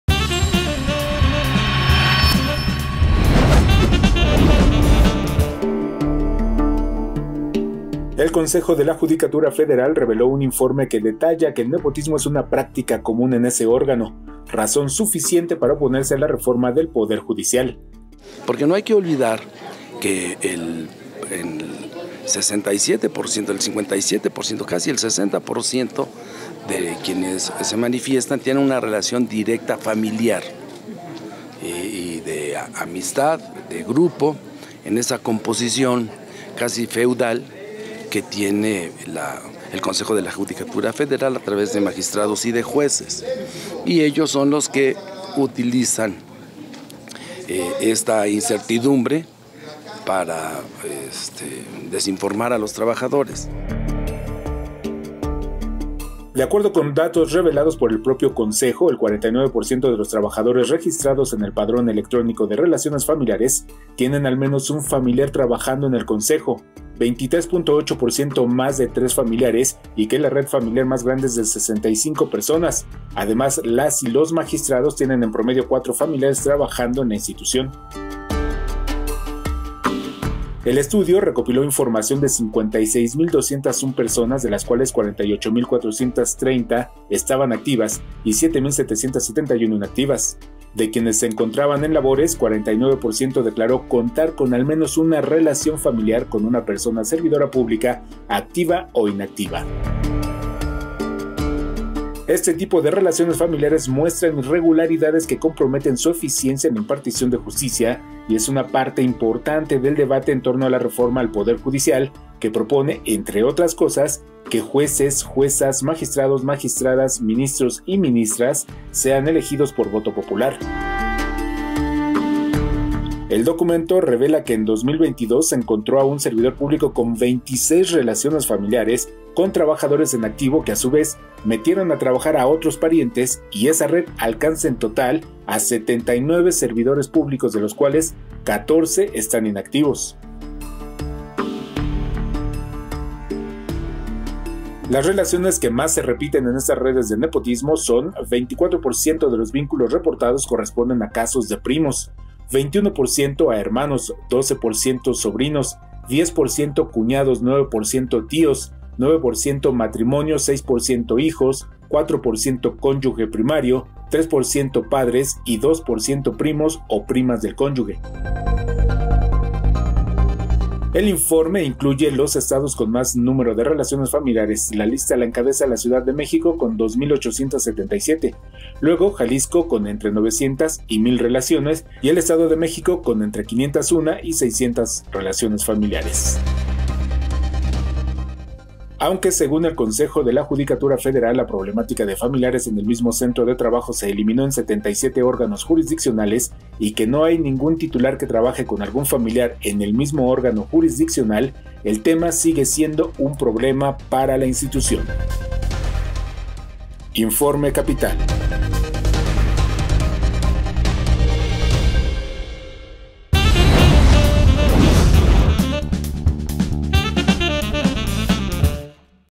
Radiografía sobre el Nepotismoen el Poder Judicial.
📌El Consejo de la Judicatura Federal reveló un informe que detalla que el nepotismo es una práctica común en ese órgano, razón suficiente para oponerse a la reforma del Poder Judicial.

InformeCapital con Braulio Luna. 💬📰